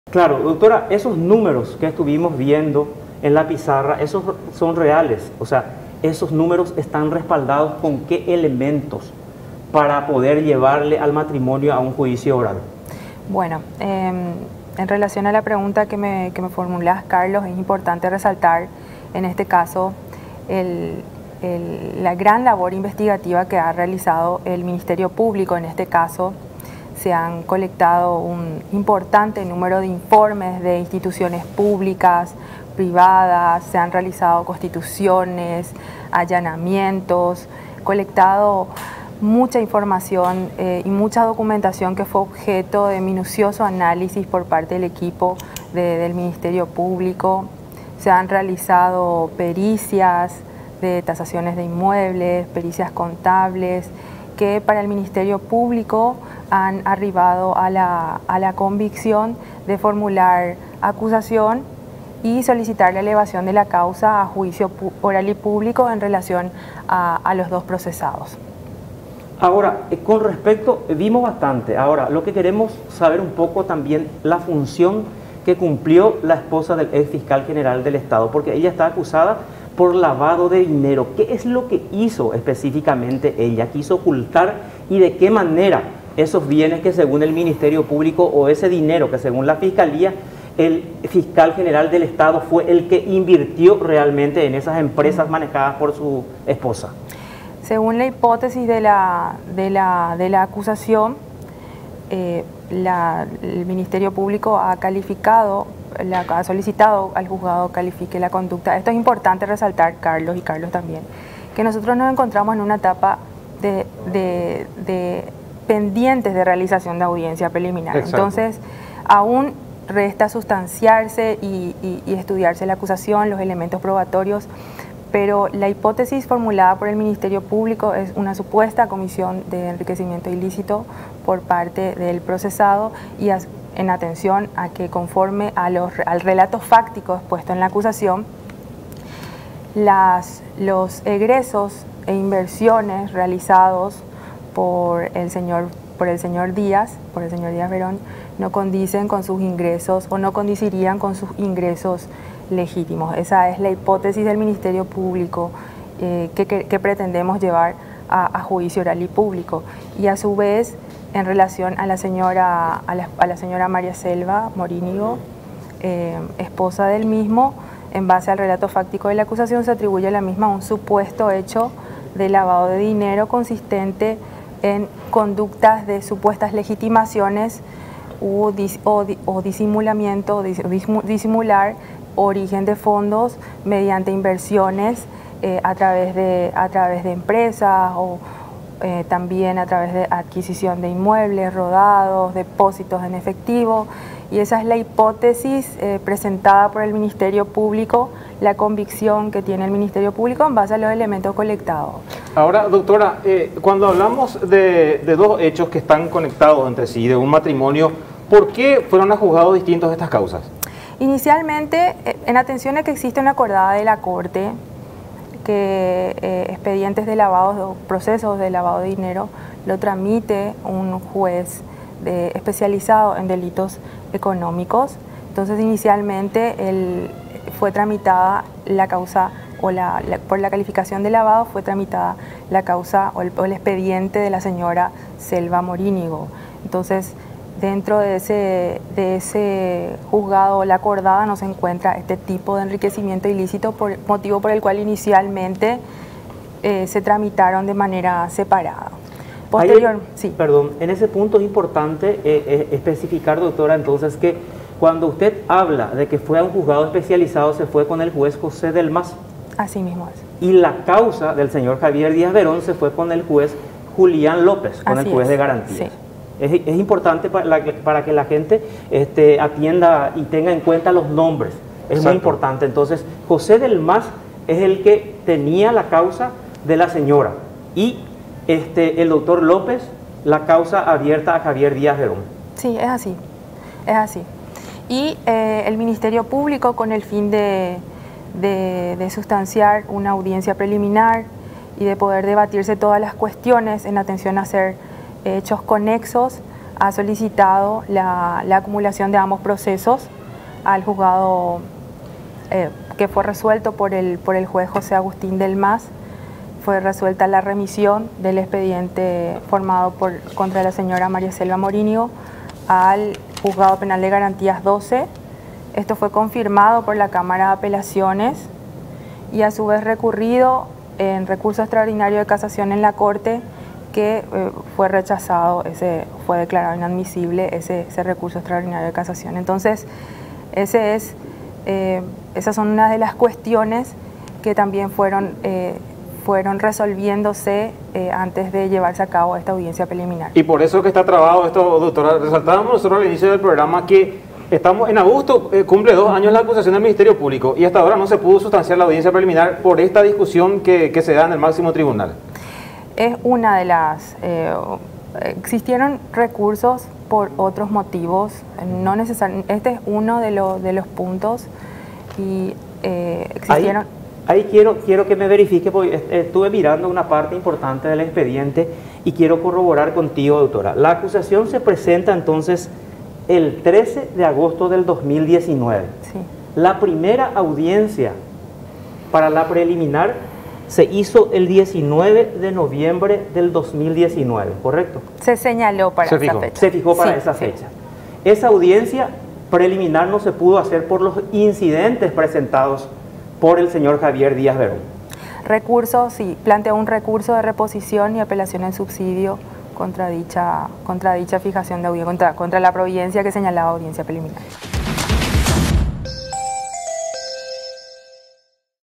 Fiscalía busca llevar a juicio oral a Javier Díaz Verón y su esposa
La fiscala Natalia Sosa habló acerca de la investigación a Javier Díaz Verón y a su esposa, y contó por qué cree que el Ministerio Público tiene todos los elementos para llevar a los acusados a un juicio oral.